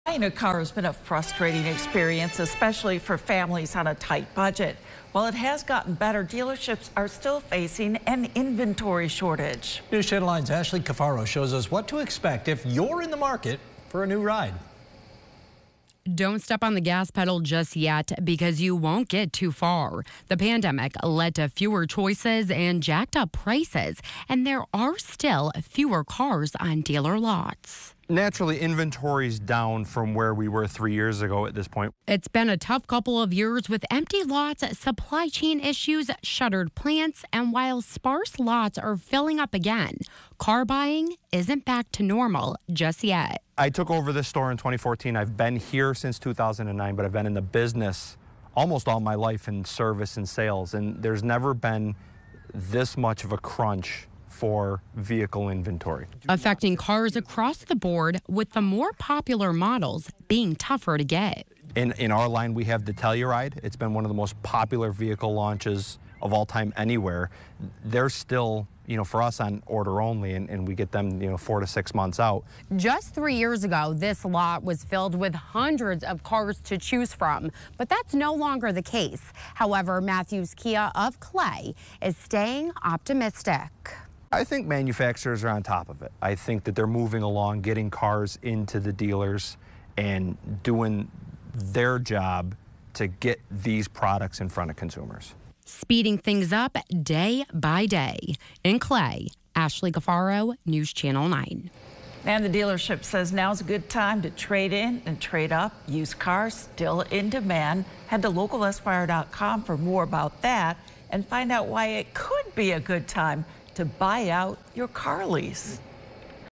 Pandemic, chip shortage still impacting car inventory
Buying a car has been a frustrating experience, especially for families on a tight budget. While its gotten better, dealerships are still facing a shortage of inventory.